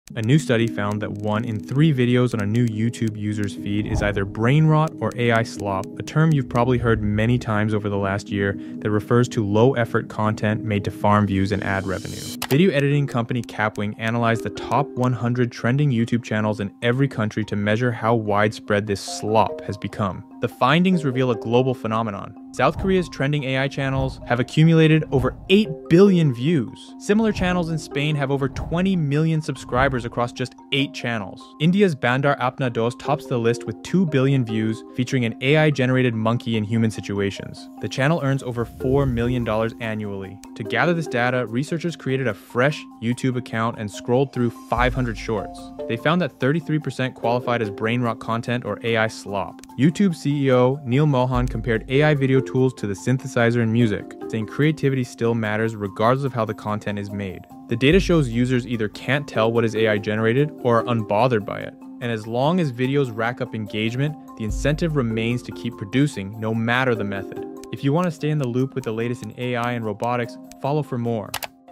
Is AI slop taking over social media?
A new study from Kapwing reveals that AI-generated content makes up a significant portion of YouTube's ecosystem.

Researchers analyzed trending channels across every country and found South Korea's AI-generated channels have amassed 8.45 billion views, while Spain leads with over 20 million subscribers.

The top channel, India's Bandar Apna Dost, has earned an estimated $4.25 million annually from 2 billion views.

When researchers created a fresh YouTube account, 21 percent of the first 500 recommended Shorts were AI-generated, and 33 percent qualified as low-quality brainrot content.

The findings highlight how algorithmically-generated videos now compete directly with traditional creator content for audience attention.

Study: “AI Slop Report: The Global Rise of Low-Quality AI Videos” by Kapwing